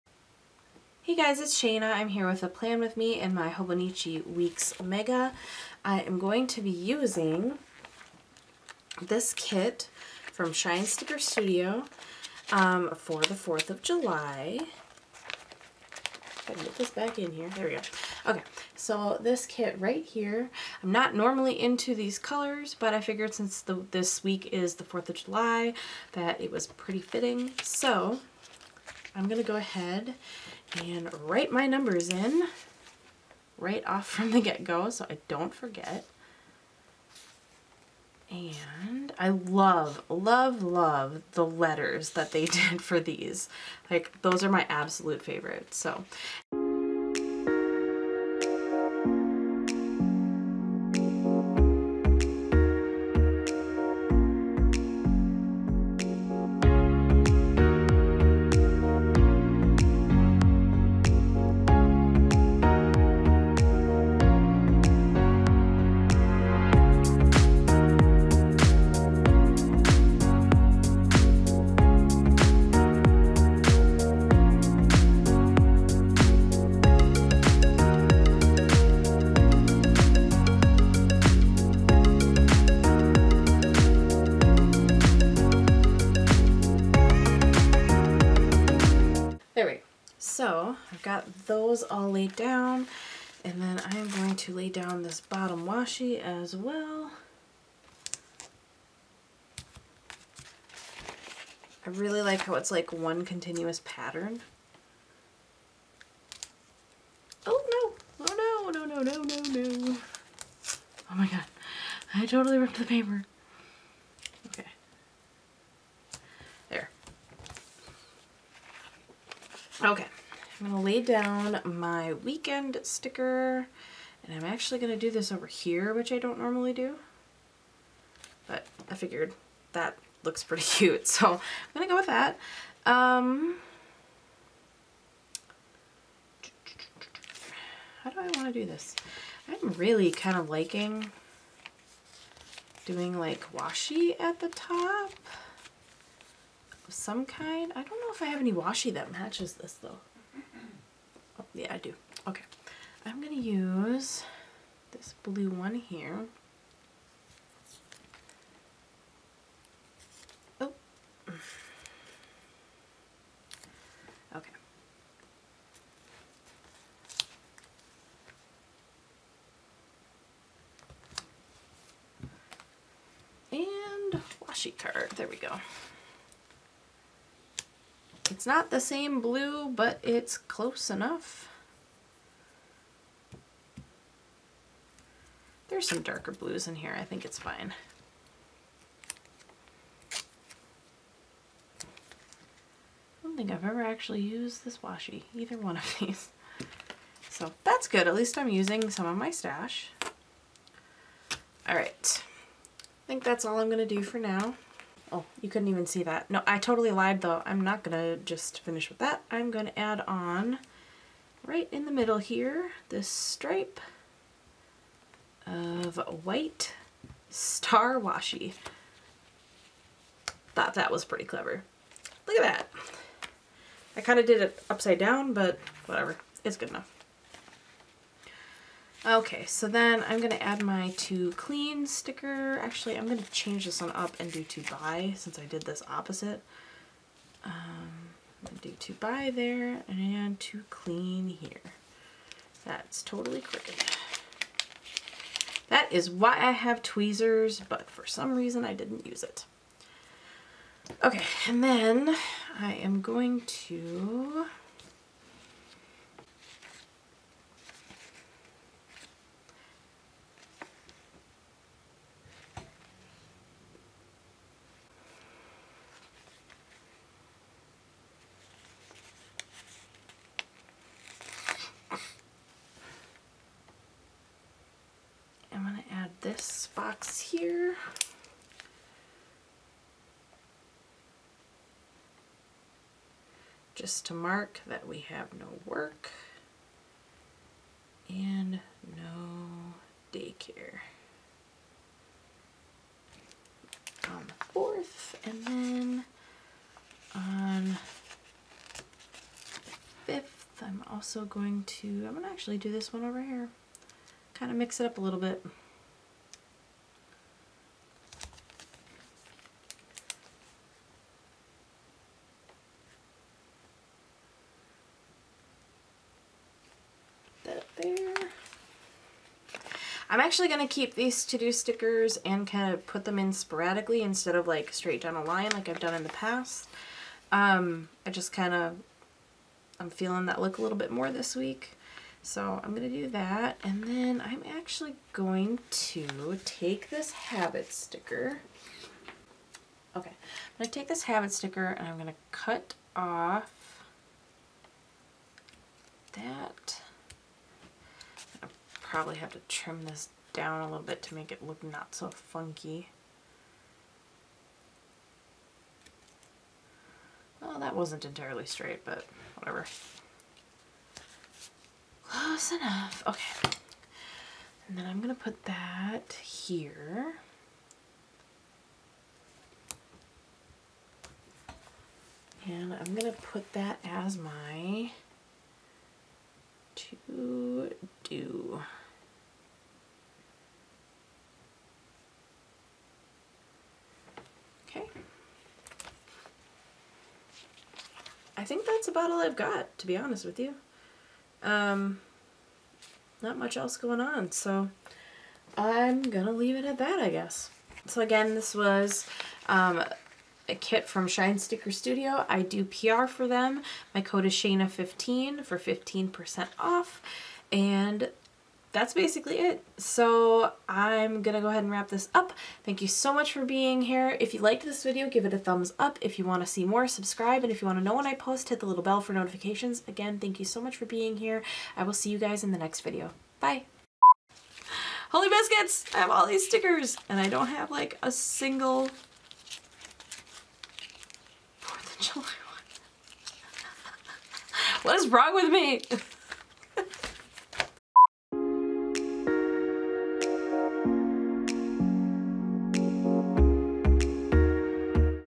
Hobonichi Plan With Me // July 1 - 7 // ft. Shine Sticker Studio
July 1 - 7, 2019 Hobonichi Mega Weeks weekly setup using the 4th of July kit from Shine Sticker Studio.